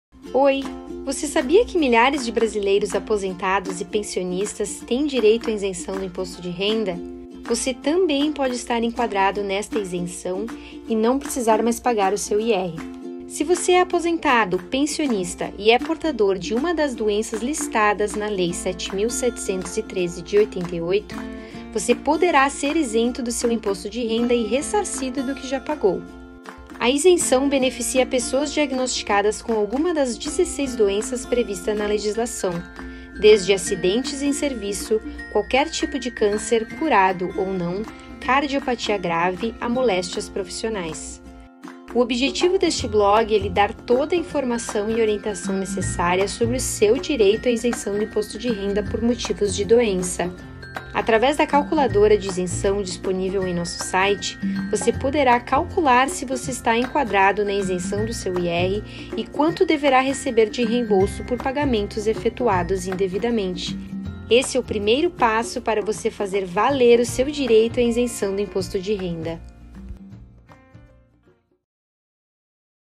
COMO SER ISENTO DO IMPOSTO DE RENDA
Se você é aposentado/pensionista e é portador de uma das doenças listadas na LEI 7.713/88, você poderá ser isento do seu imposto de renda e ressarcido do que já pagou.

Clique no link abaixo e confira se você está pagando imposto indevidamente e quanto você poderá receber de reembolso.